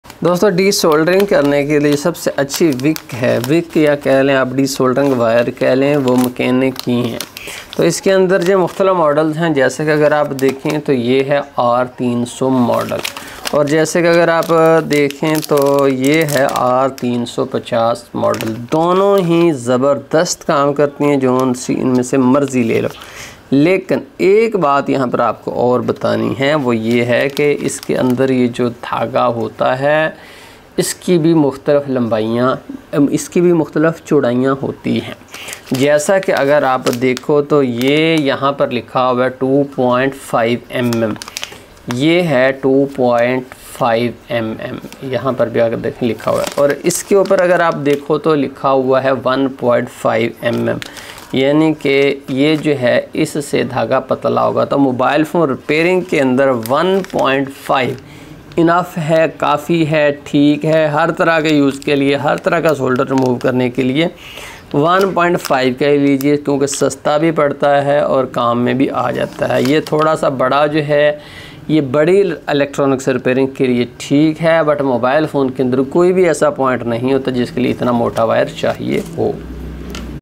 MECHANIC R300 Solder Wick 1.5mm Desoldering Braid Wire Tin Remover BGA Welding Soldering Tools
Product details of MECHANIC R300 Solder Wick 1.5mm Desoldering Braid Wire Tin Remover BGA Welding Soldering Tools
This desolder wick is used for removing excess solder from Printed circuit board you are working on. The extended metal tip allows you to use this without risking burning your hand from being too close to the soldering iron.Width: 2.0mm Length: 1.5mScope of application: Tin removalMaximum suction: 30
Brand : MECHANIC R300Material: Copper.Type : Desoldering Braid.Length: 1.5 mWeight: about 8 gWidth : 1.5mmFeature : Anti-oxidation, anti-corrosion.Function : Used to remove excess solder.
Pure oxygen free copper wire, no clean flux for easy to use. The solder wick can help clean some residue flux and BGA thermal bonding pad too.
- Wide Range of Uses.Ideal for use in labs, service shops, schools (STEM - steam), home and industry welding work.
Specifications of MECHANIC R300 Solder Wick 1.5mm Desoldering Braid Wire Tin Remover BGA Welding Soldering Tools